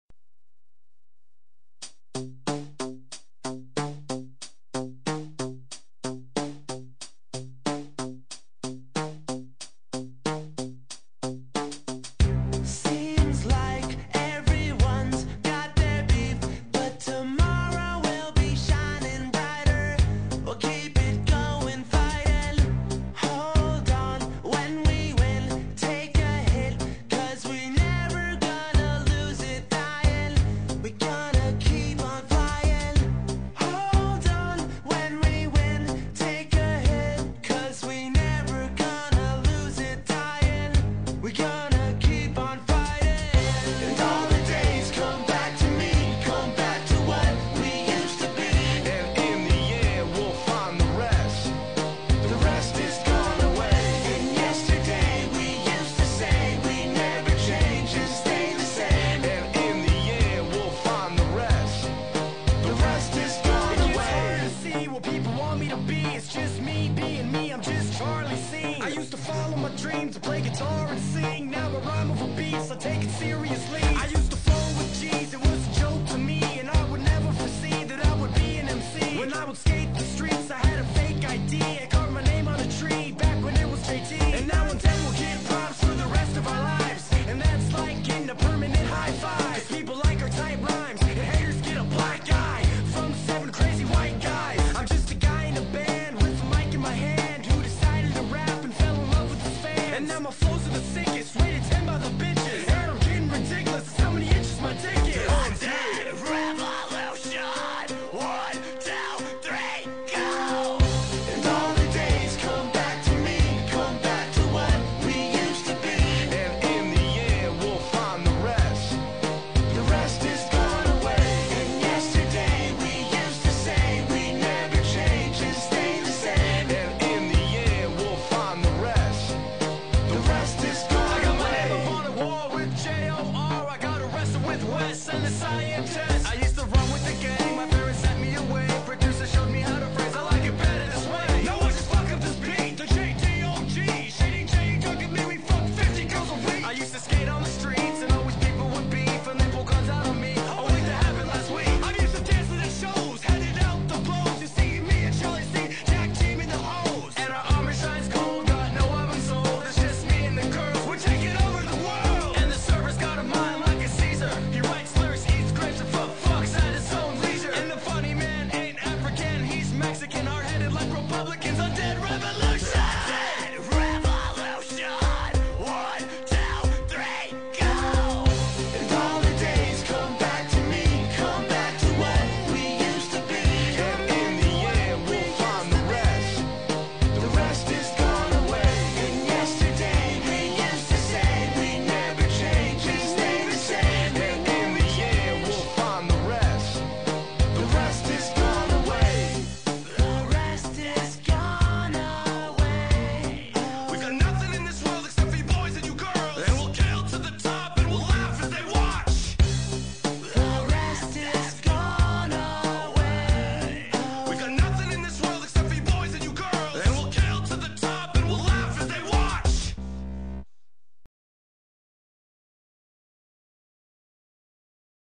How to make a song instrumental
How to make a song instrumental using audacity to download audacity go